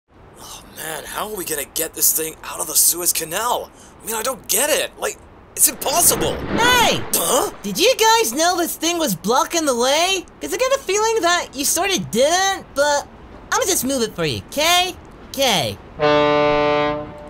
What If Goku Cleared The Suez Canal?
What If Goku could free the Ever Given Blocked in the Suez Canal using his skills?

Original art done by Whirlydoodle